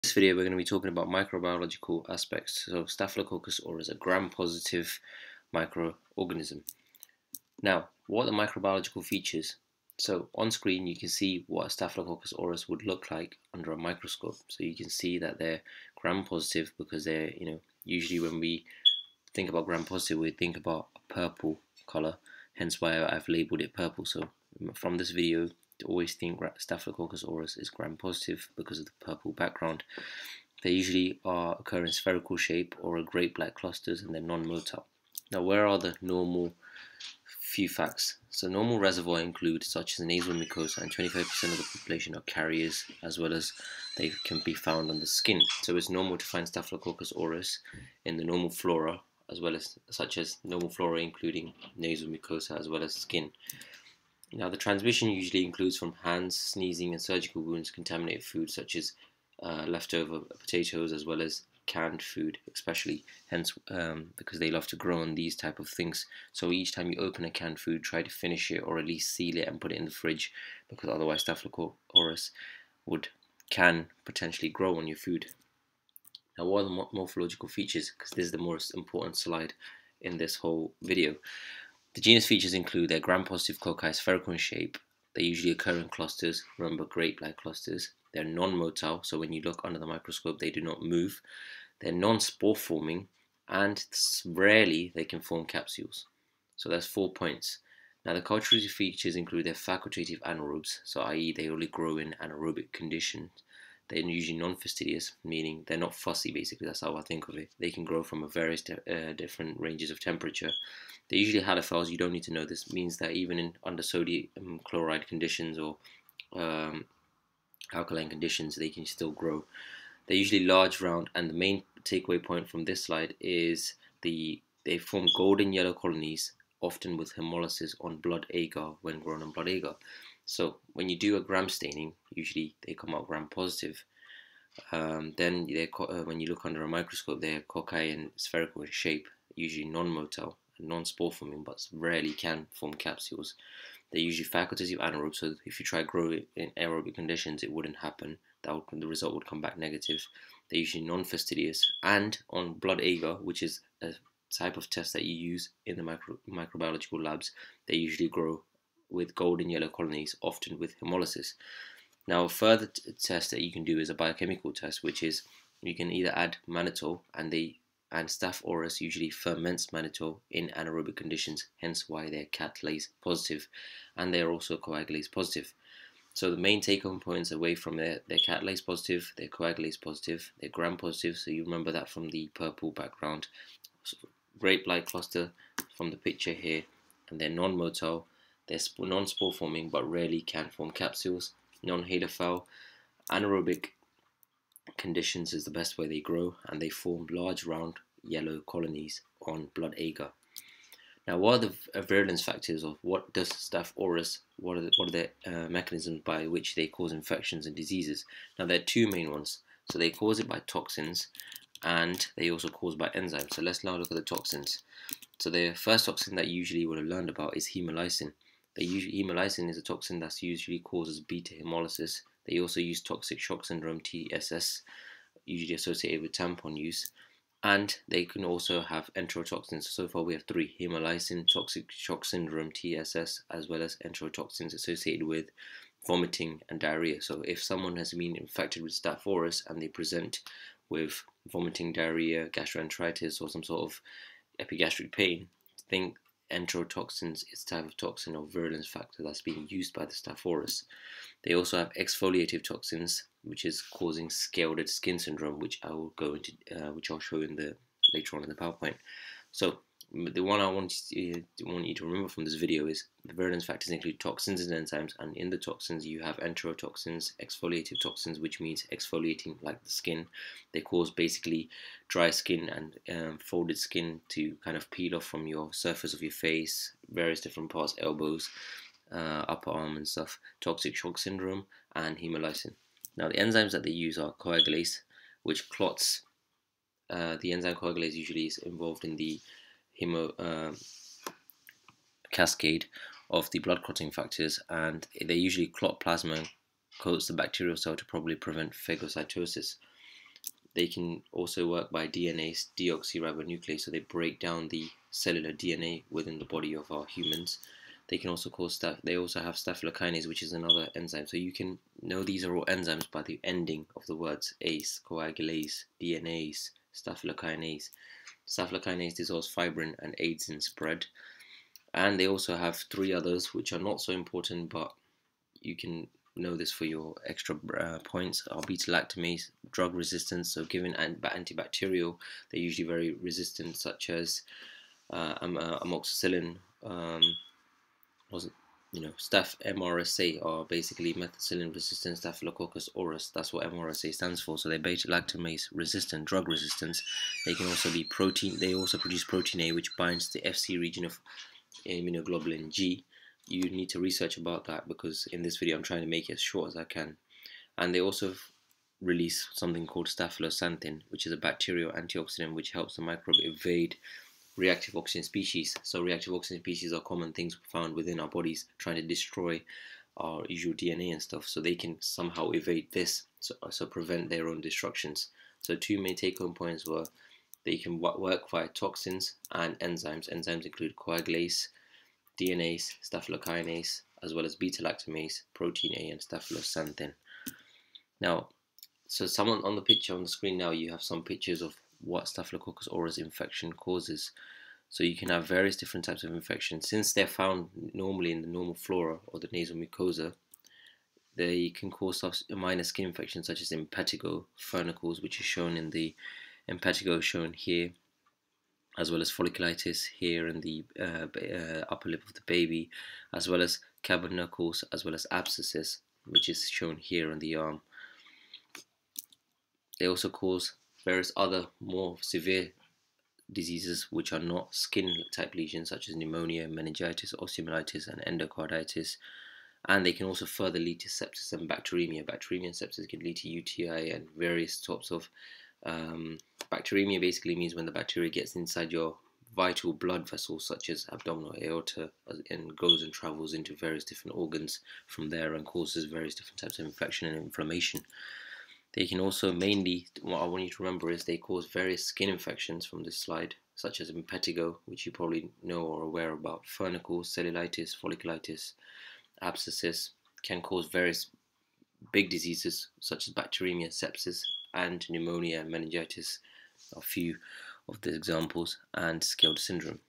Microbiology - Staphylococcus Aureus : morphology, genus features, pathology
everything you need to know about staph aureus , gram positive Genus Features: Gram+ (“Moses wearing violet robe”), cocci in clusters, catalase+ (“cat”), small, yellow colonies on blood agar,
Beta-hemolytic (“bright light with Beta”), Coagulase+ (“blood red sea parting”), Ferments mannitol on mannitol salt agar (“Tall-
man with yellow clothes”)
 Reservoir: Normal flora – nasal mucosa; 25% of population are carriers (“Sphinx’s nose in the foreground”), and skin
 Transmission: hands, sneezing, surgical wounds, contaminated foods – i.e. custard pastries, potato salad, canned meats
(“meats and mayonnaise left out too long hanging from camel with nauseous woman”)
 Predisposing Factors for Infection: surgery/ wounds, foreign body – tampons, surgical packing, sutures – severe neutropenia, IV drug abuse, chronic granulomatous disease, Cystic Fibrosis
 Disease and Pathogenesis:
• Protein A binds Fc component of IgG, inhibits phagocytosis = “Moses’ staff with ‘A’ binding to antibody at Fc”